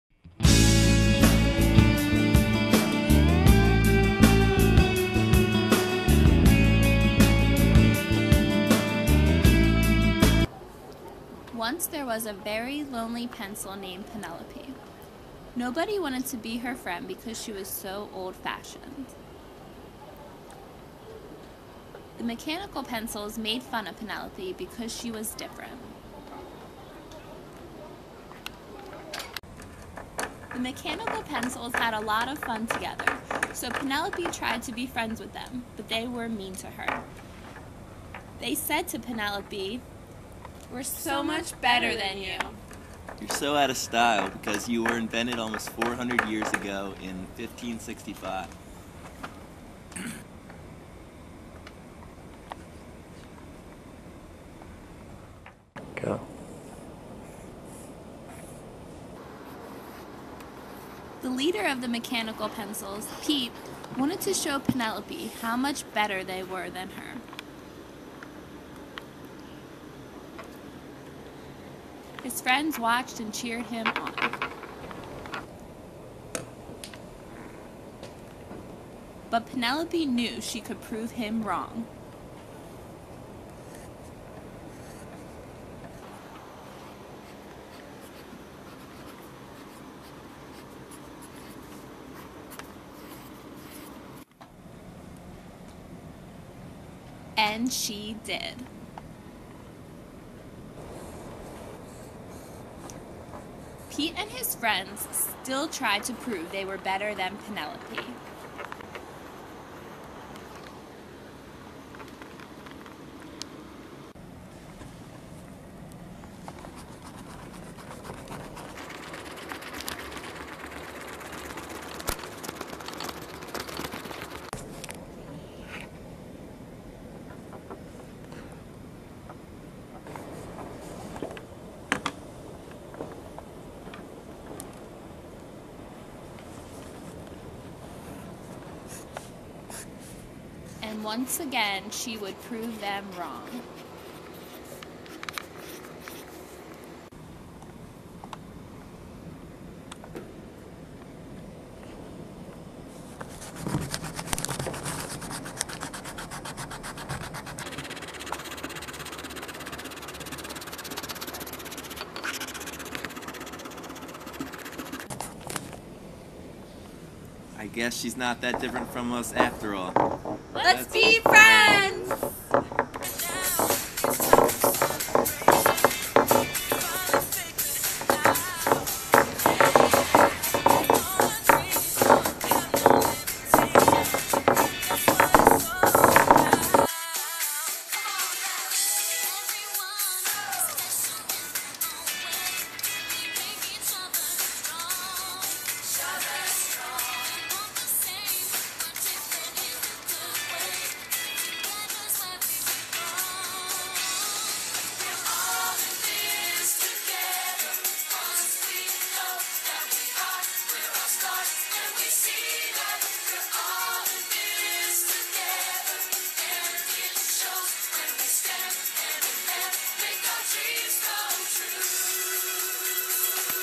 Pencil Pals
Adapted from the novel "Wicked," the class rewrote the book into a children's story, then adapted again as a movie.